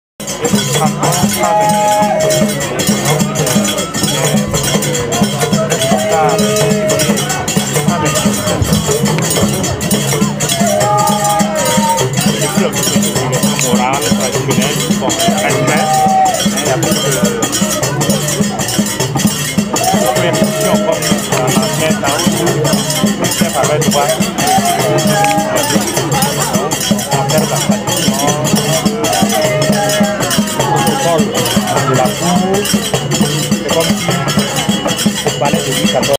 Manega, Burkina Faso
Fete du chef du village. Mission canadienne VOIR AUTREMENT